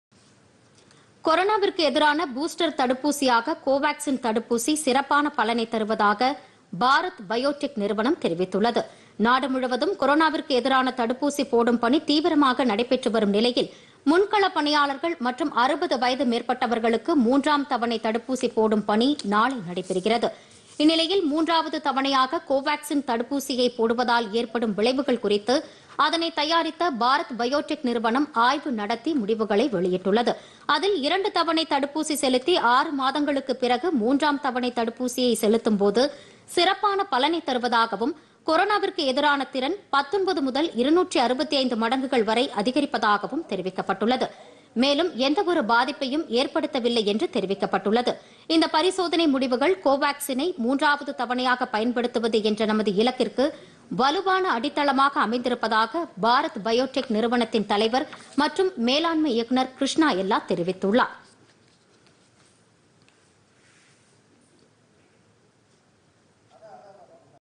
சிறப்பான பலனைத் தரும் கோவேக்சின்
மூன்றாவது தவணை  தடுப்பூசியாக கோவேக்சின் சிறப்பான பலனைத் தருகிறது - பரிசோதனை முடிவுகளில் தகவல்.